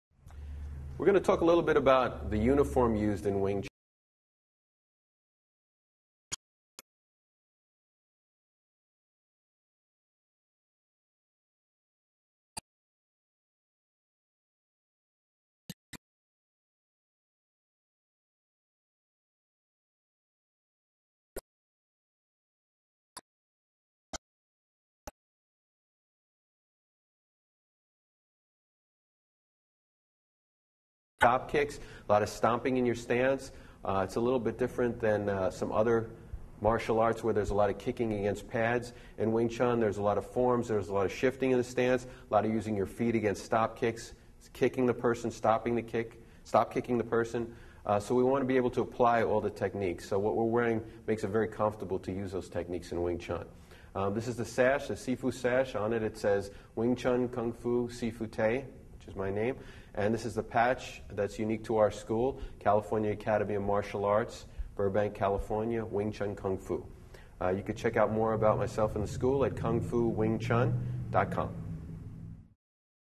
Wing Tsun Uniform
Wing Tsun Uniform. Part of the series: Beginning Wing Tsun Techniques. The Wing Tsun uniform is a unique form of attire worn for this type of Kung Fu. Learn more about the uniform from a professional martial artist in this free video.